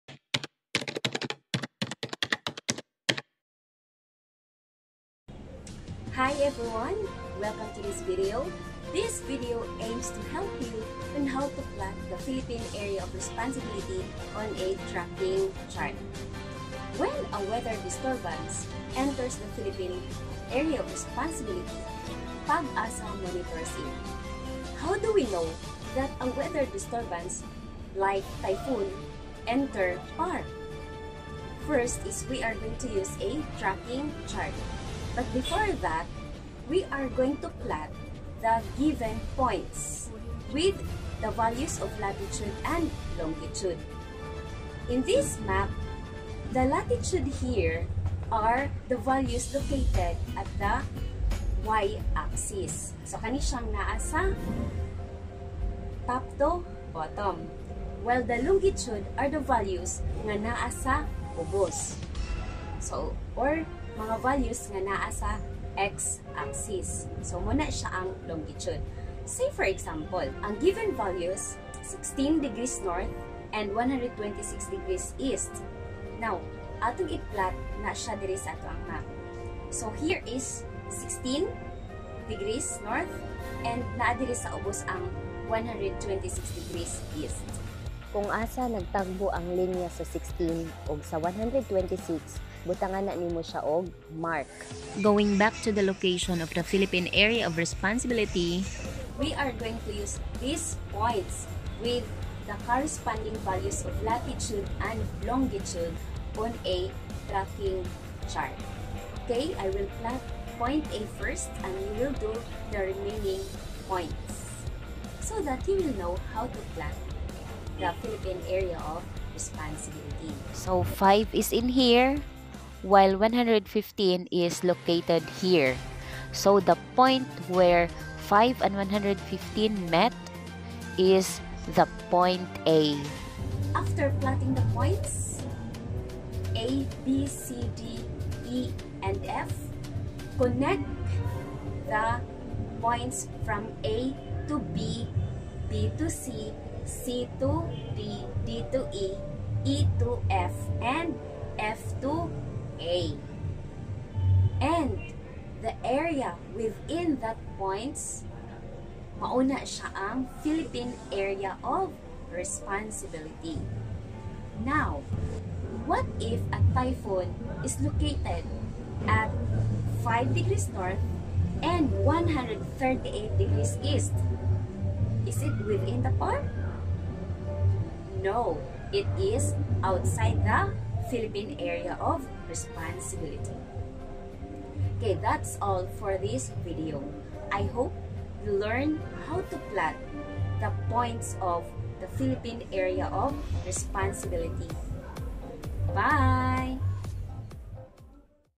Plotting the (PAR) Philippine Area of Responsibility
guide on how ro plot the PAR